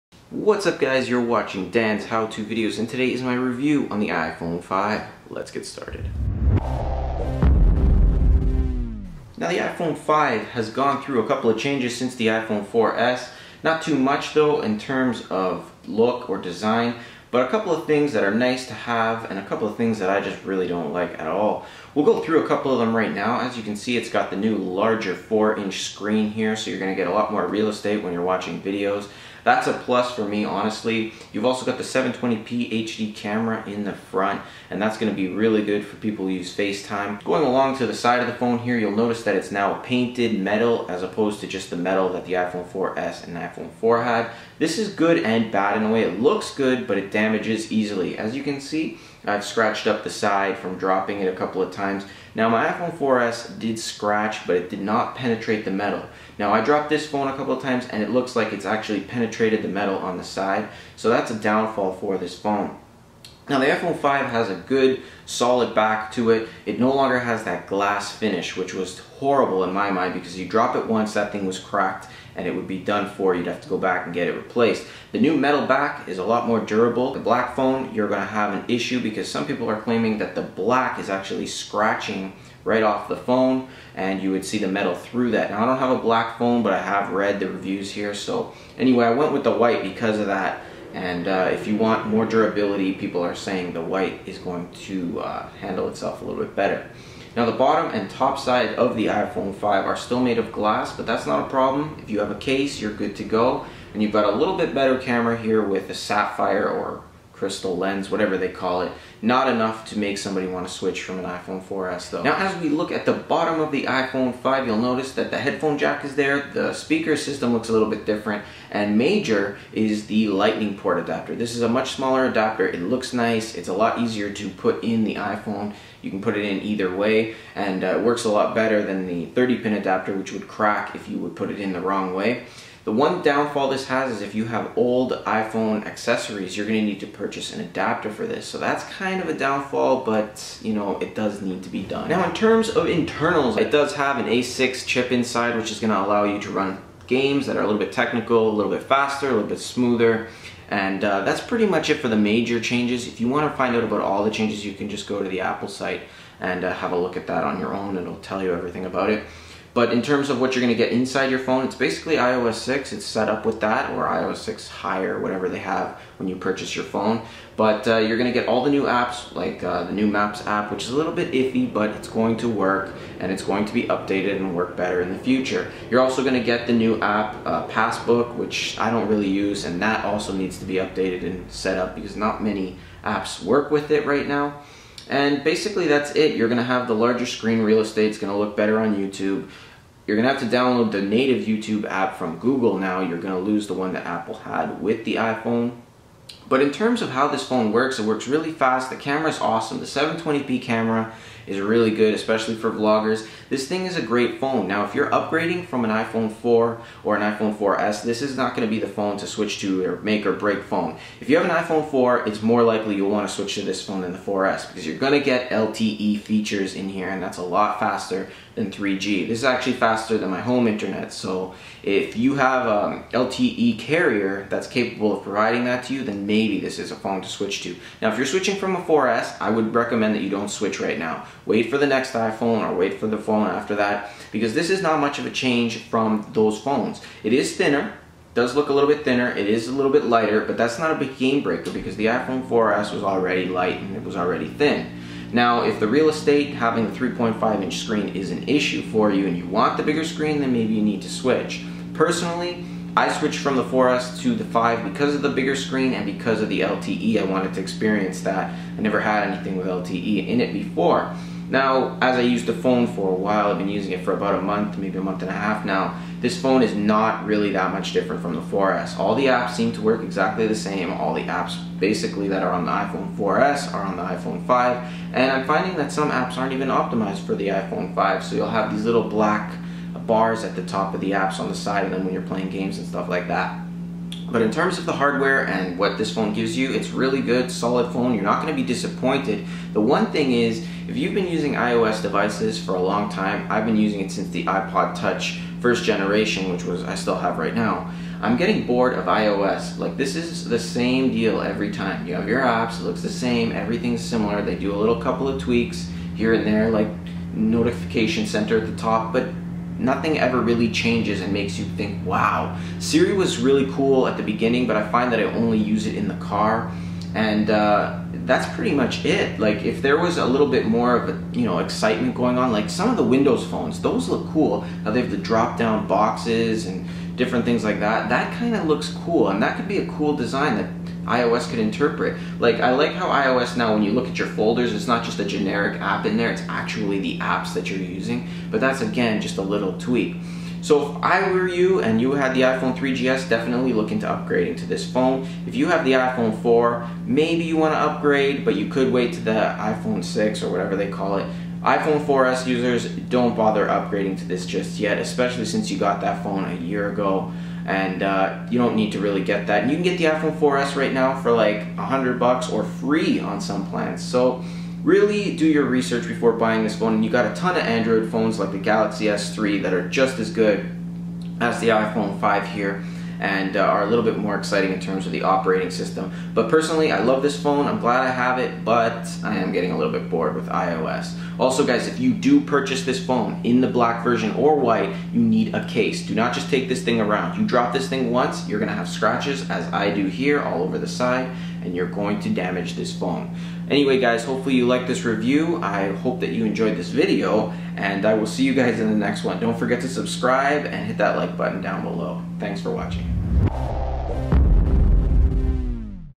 iPhone 5 Review - Should You Upgrade To The iPhone 5?
This is a full unbiased review of the iPhone 5. Should you upgrade to the iPhone 5? This video will help you decide.